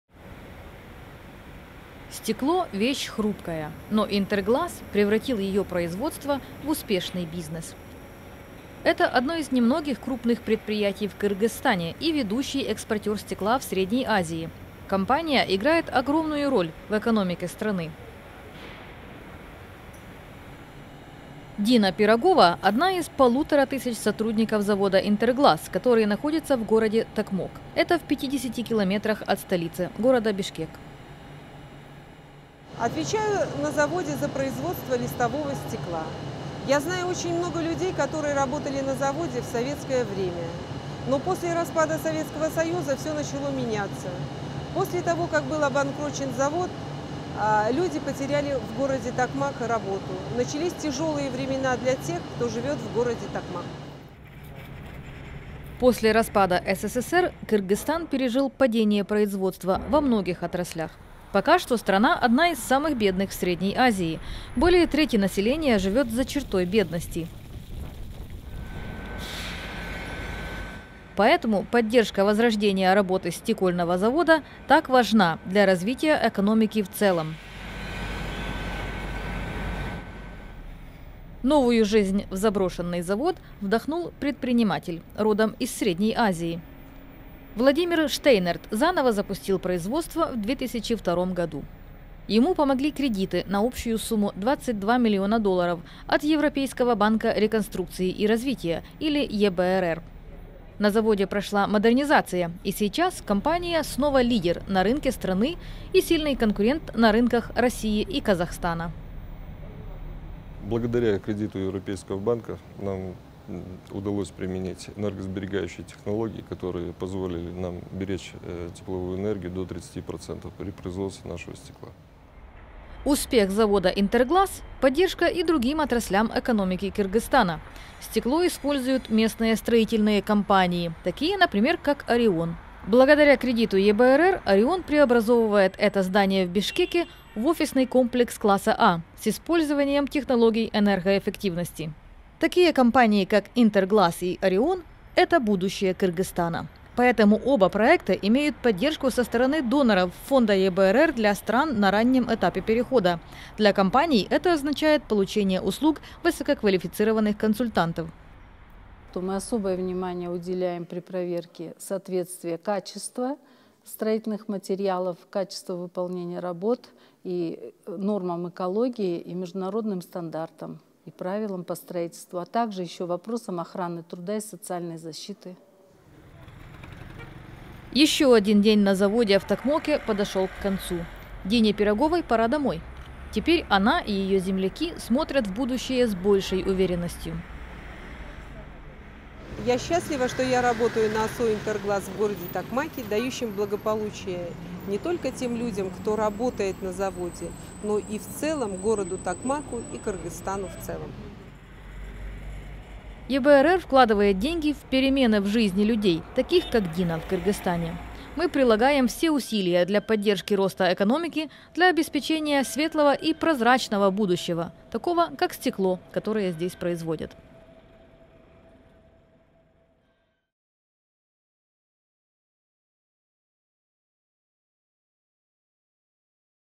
Поддержка частного сектора в Кыргызстане
ЕБРР и страны-доноры помогают вдохнуть новую жизнь в самую большую кыргызскую компанию-производителя стекла. Это помогает кыргызской экономике, создает рабочие места и стимулирует строительный сектор. Поддерживается Фонд, предназначенный для стран, находящихся на раннем этапе переходного периода.